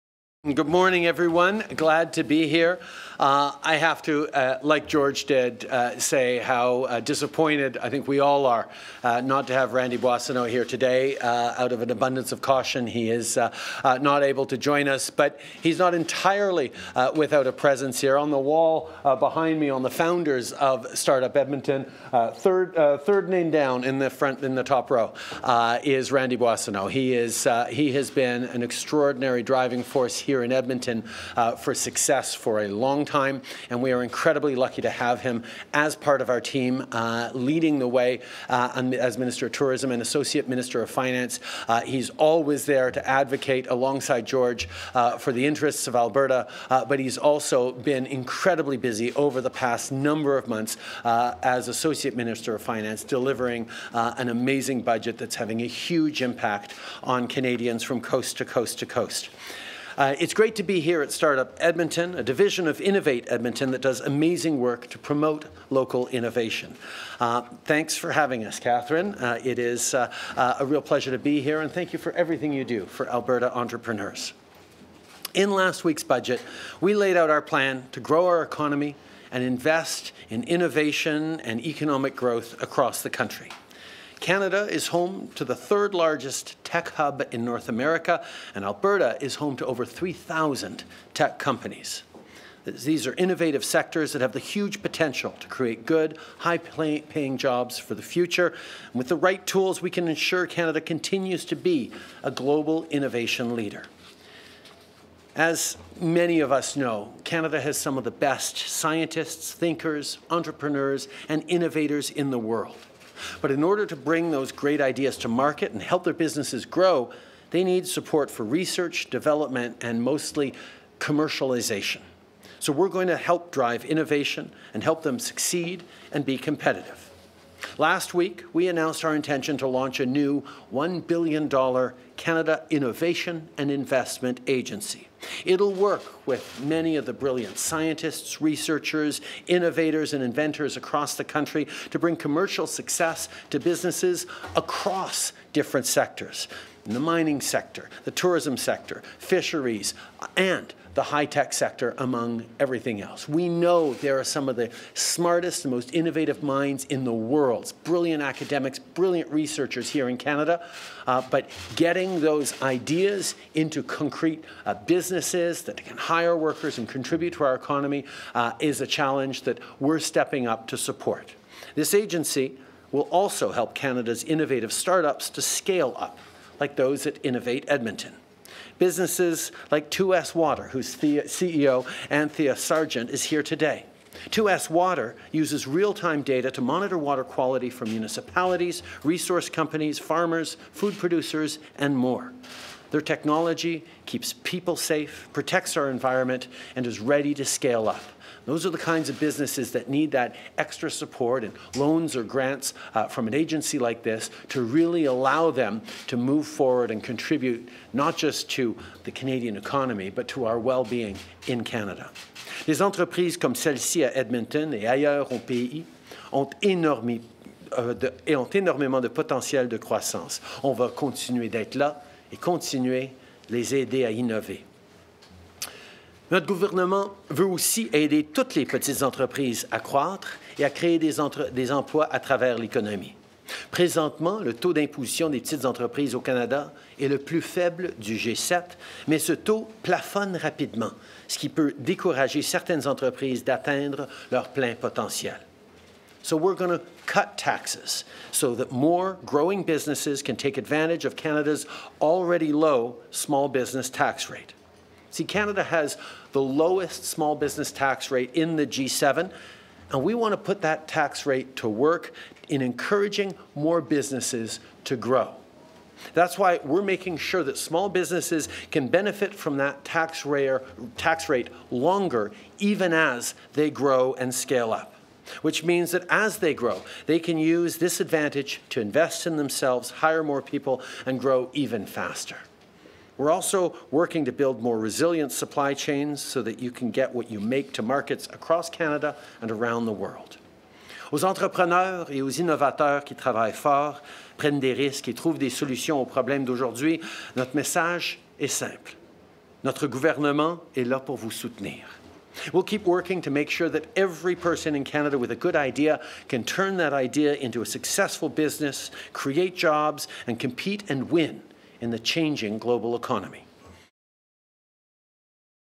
Supporting small businesses and creating good jobs
Prime Minister Justin Trudeau makes an announcement highlighting support for small businesses in Budget 2022.